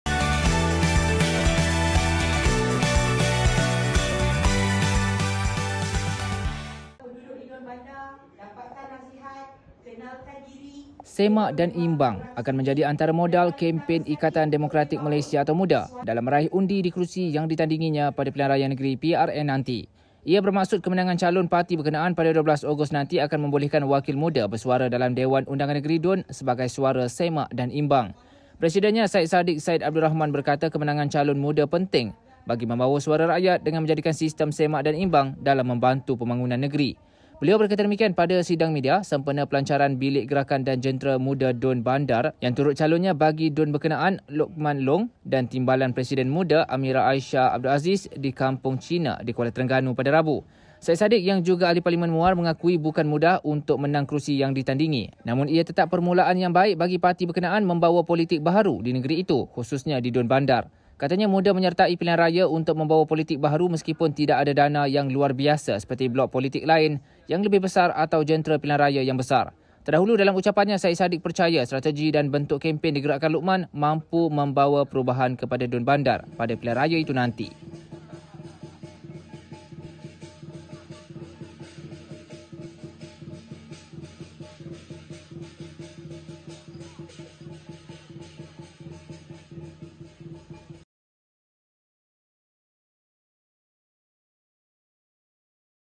PRN: 'Semak dan imbang' modal kempen MUDA
'Semak dan imbang' akan menjadi antara modal kempen Ikatan Demokratik Malaysia (MUDA) dalam meraih undi di kerusi yang ditandinginya pada Pilihan Raya Negeri (PRN) nanti.

Ia bermaksud kemenangan calon parti berkenaan pada 12 Ogos nanti akan membolehkan wakil MUDA bersuara dalam Dewan Undangan Negeri (DUN) sebagai suara 'semak dan imbang'.